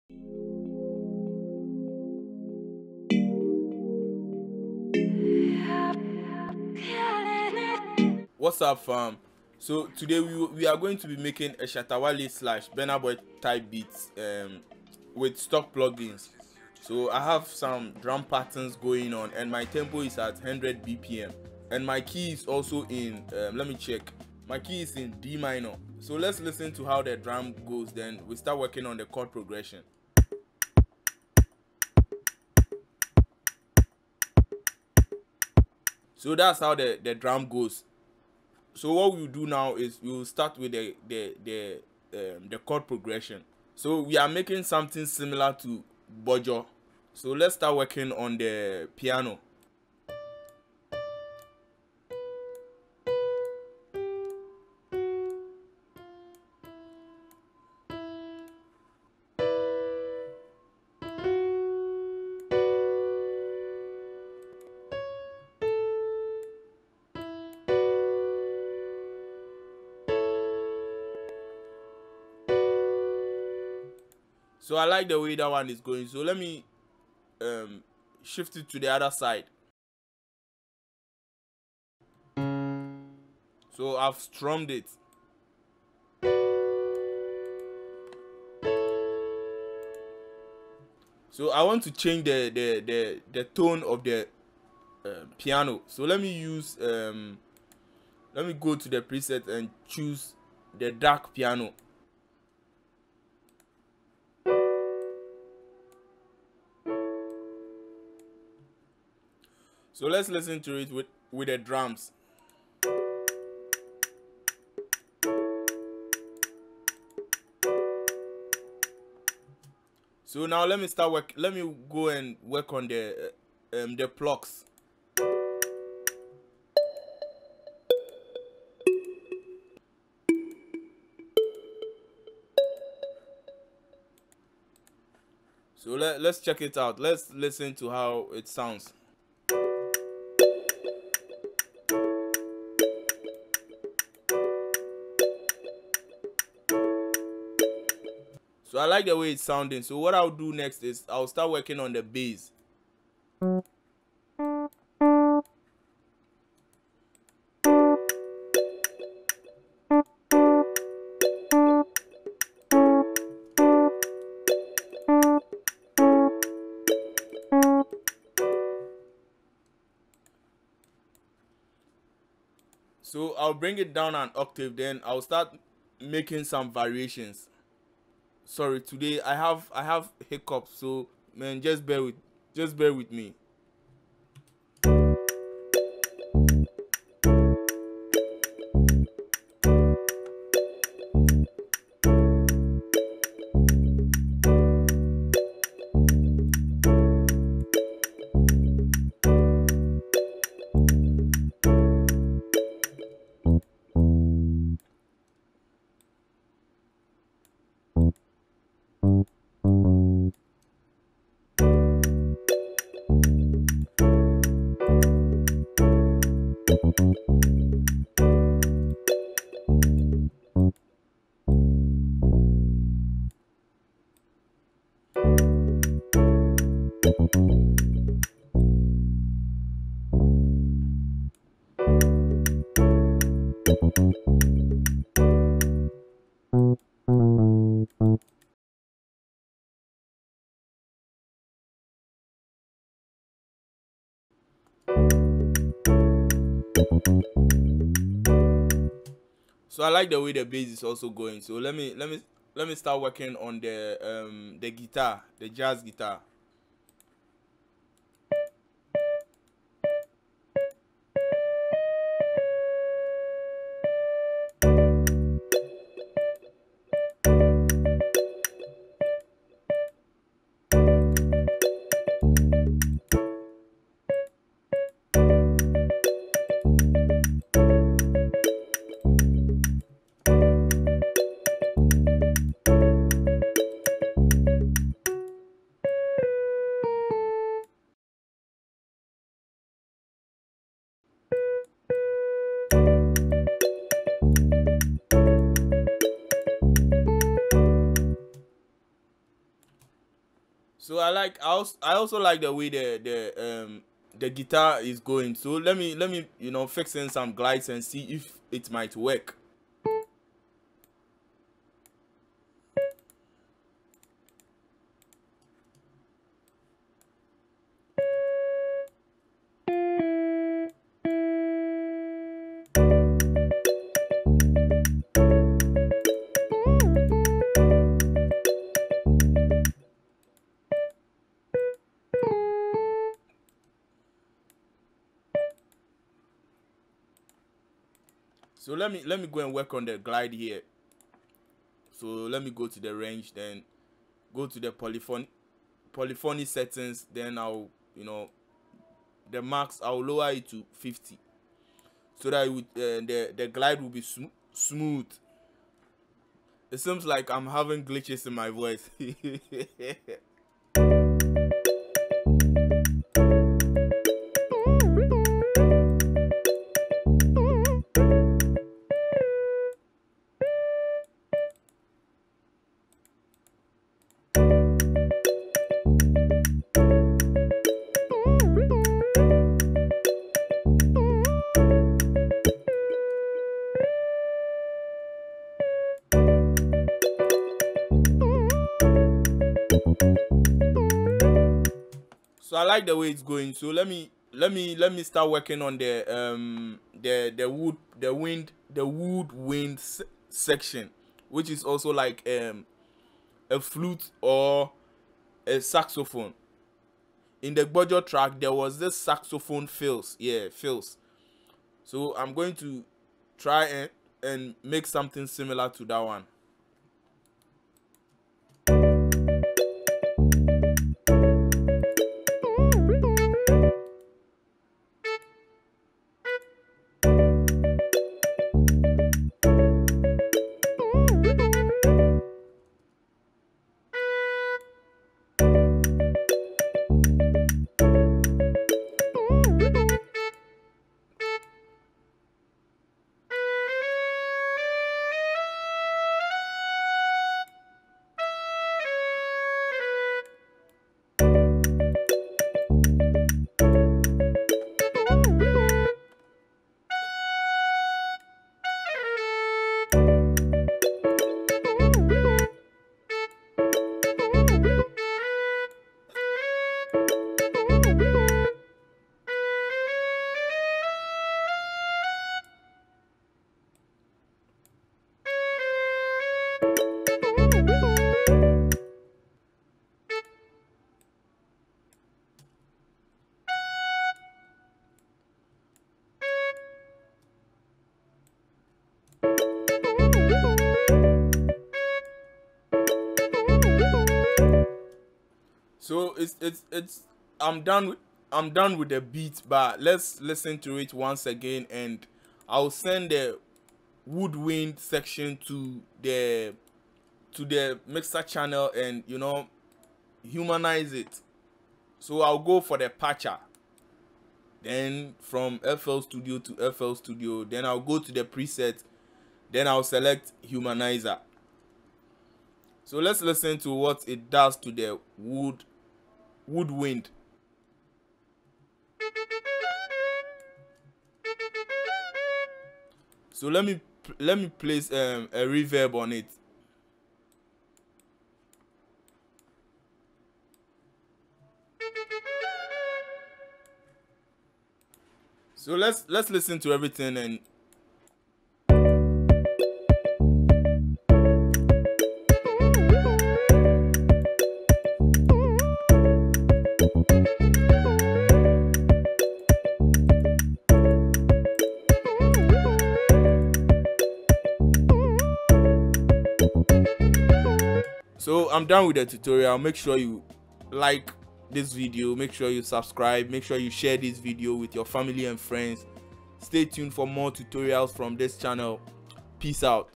How To Make Shatta Wale | Burna Boy Type Beat With Stock Plug-In 2021 Fl Studio Afro Beat Tutorial
How To Make Shatta Wale | Burna Boy Type Beat With Stock Plug-In 2021 Fl Studio Afro Beat Tutorial 2021

Comment What Tutorial You Would Want Me To Do Next.